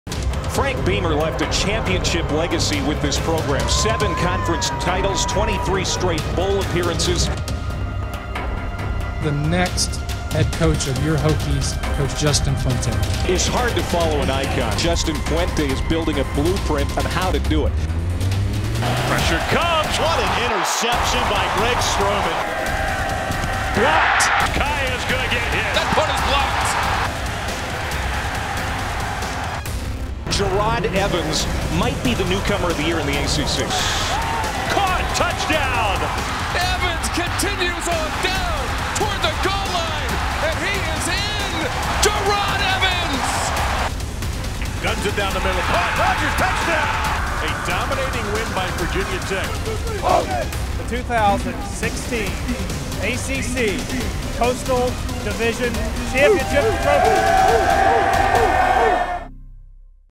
Virginia Tech Hokies Hype Video | 2016 ACC Football Championship Game
Virginia Tech is seeking its 5th ACC Championship and is set to face Clemson on Saturday in Orlando. Relive the Hokies' journey to the 2016 title game in this great Hype Video!